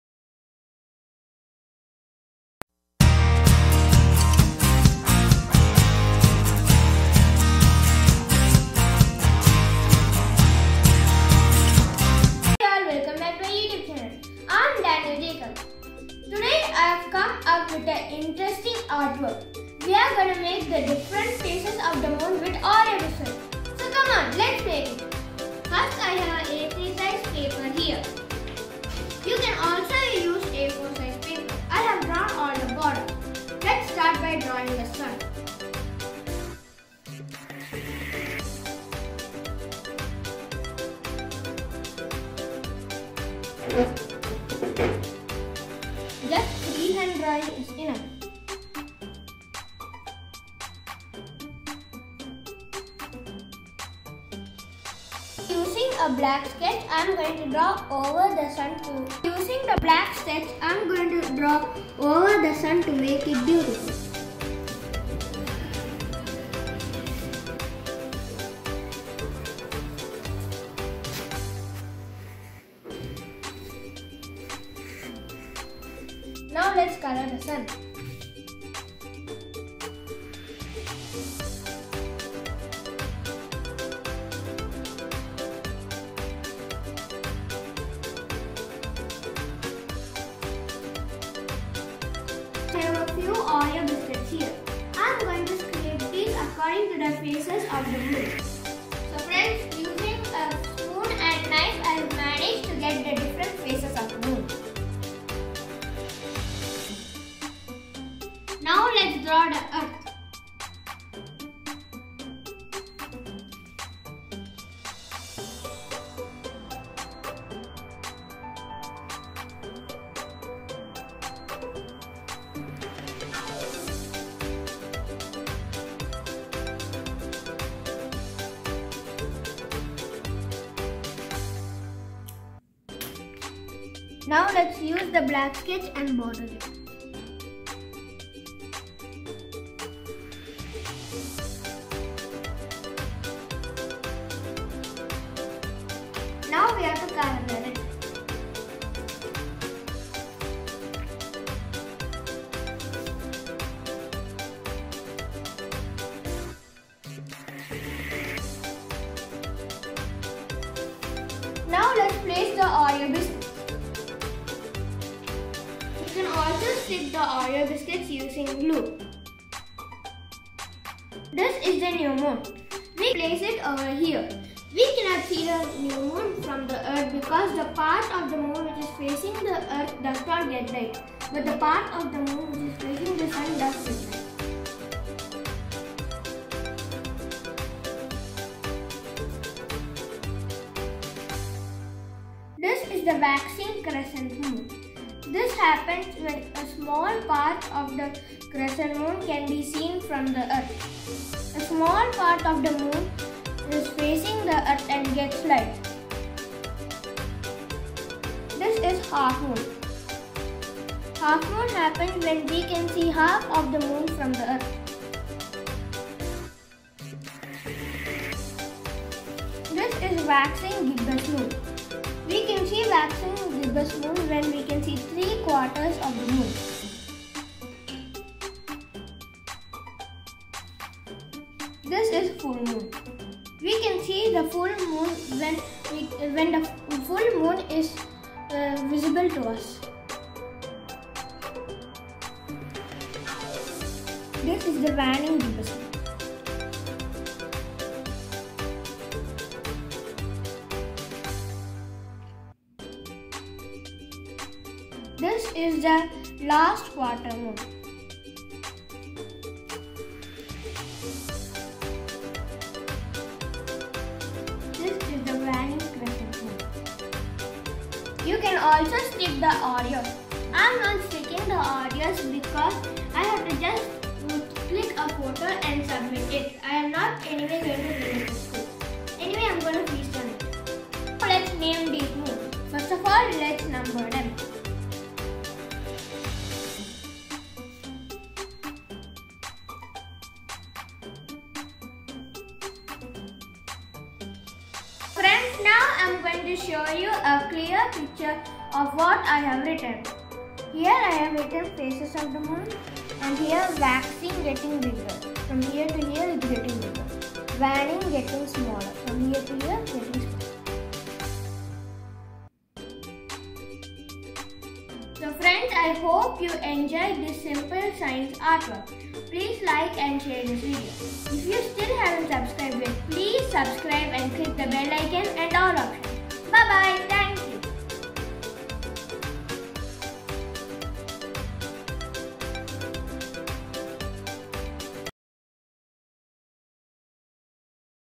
Phases Of The Moon|Fun Activity With Oreo Biscuit|Science activity for kids|Science exhibition model
Simple science acyivity that can be done usibg oreo biscuit. Kids will have a lot of fun. The different phases of the moon is also explained. Well explained and easy for kids to understand.